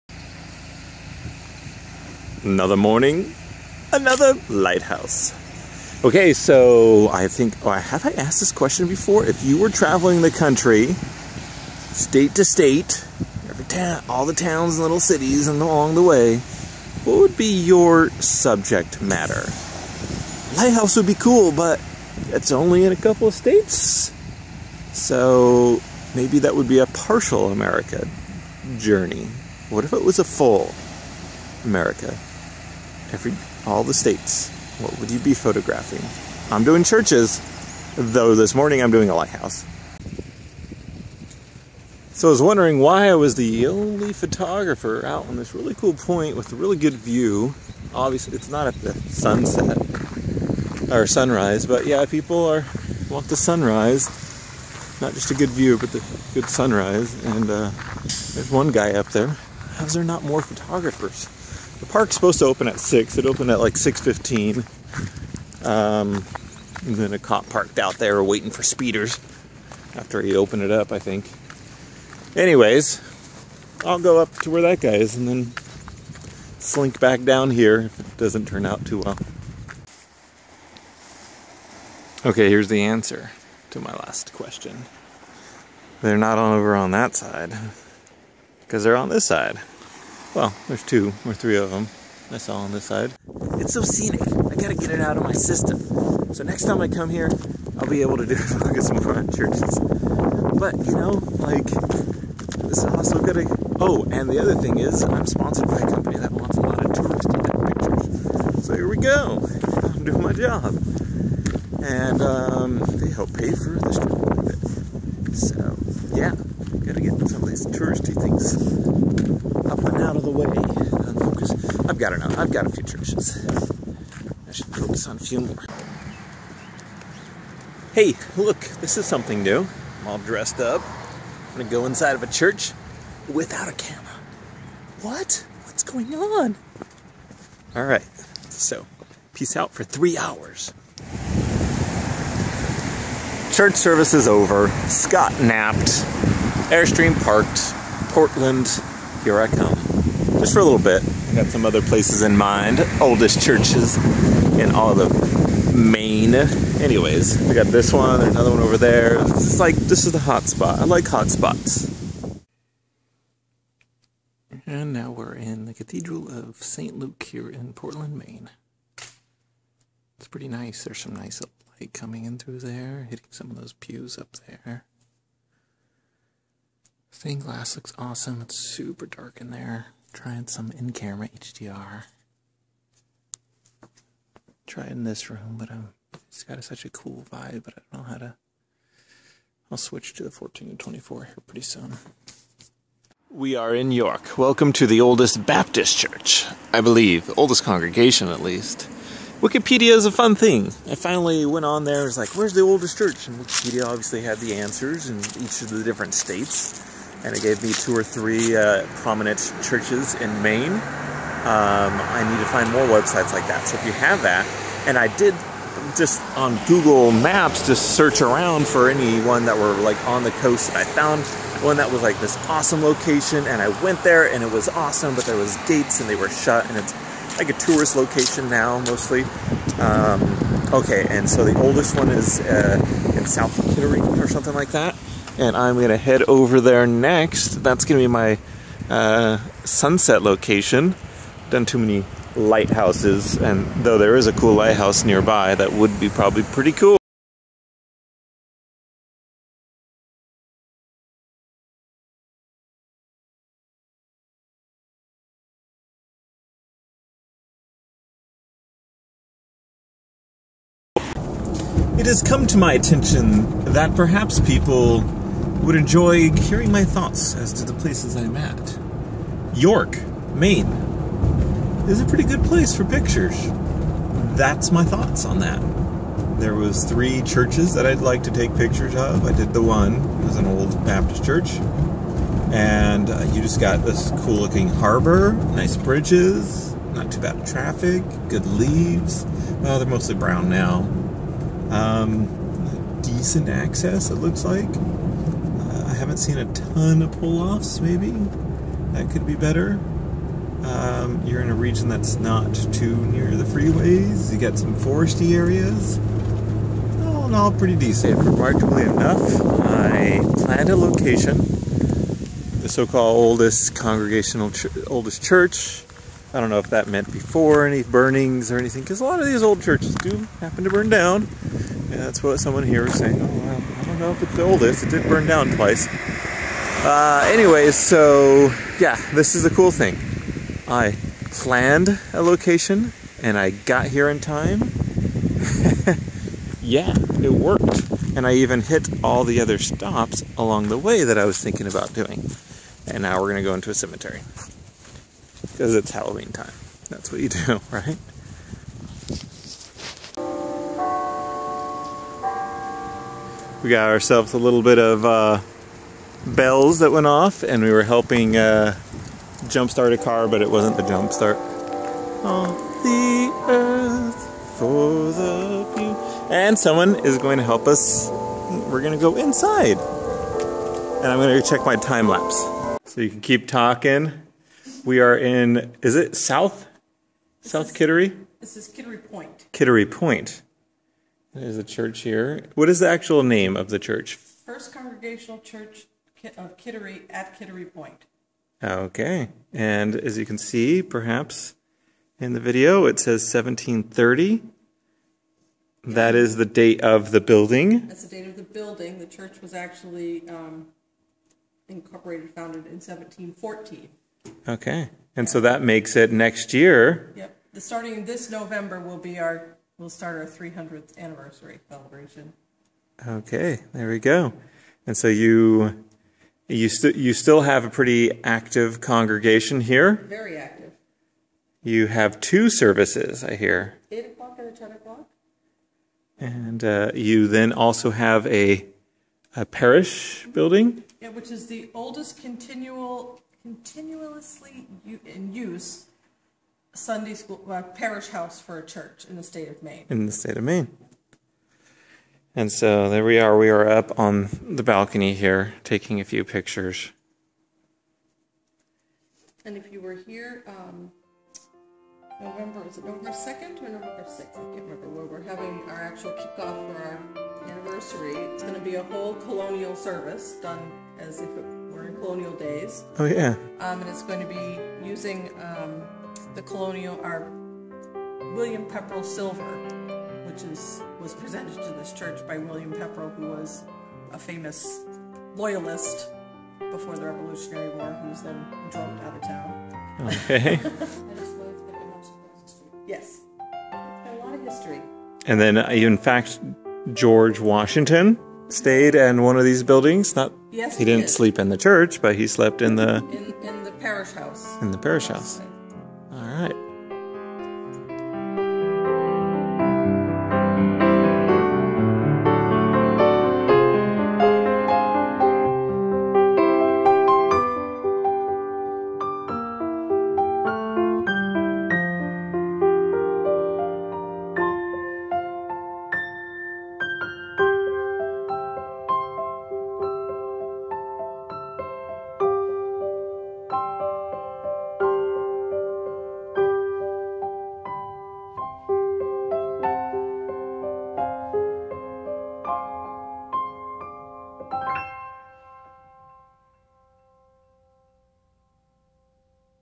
What theme would you photograph? (22)
PUNCHLINE = Portland Headlight to the oldest Maine church building
QUESTION = What subject would you focus on if you went to Every State?

DAY 21 - October 20th

ITEMS USED AND FEATURED IN VLOG
* CAMRANGER (Remote Triggering)
- Camera D800
- Recorded with Note2 (Newer)
- Picture Editing LR5
- Timelapse GoPro